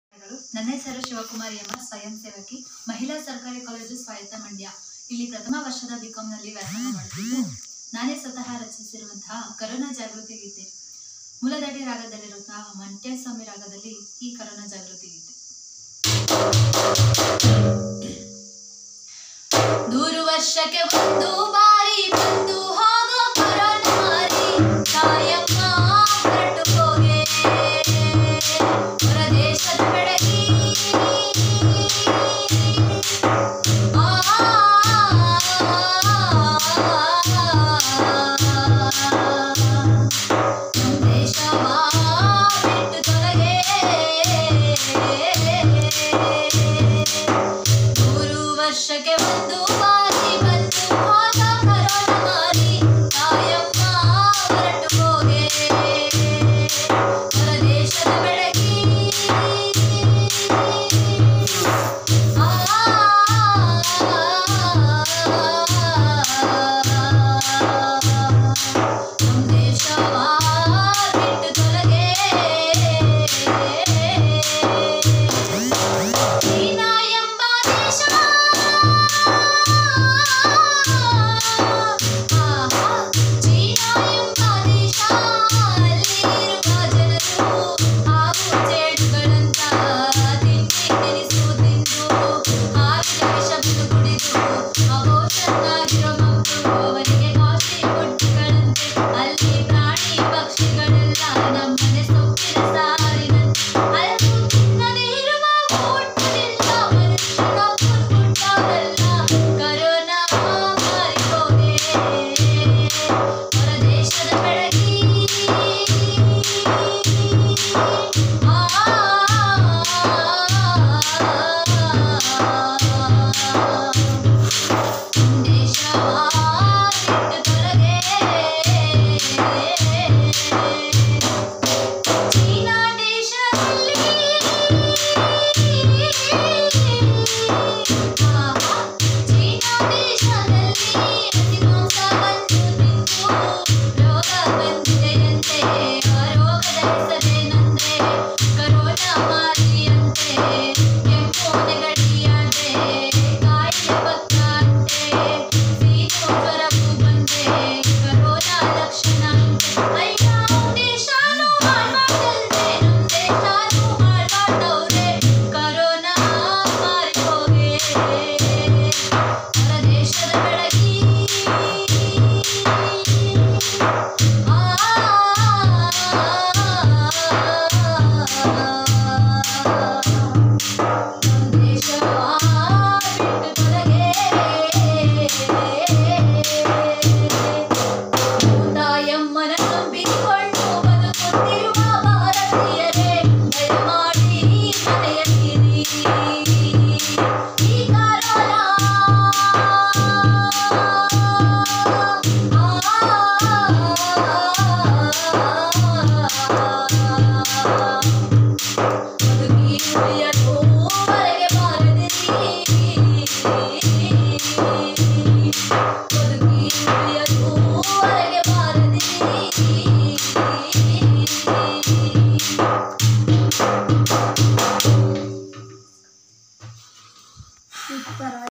Shivkumari NSS volunteer sing a song on corona virus
Shivkumari NSS volunteer Govt Degree College for Women Mandy sing a song on corona virus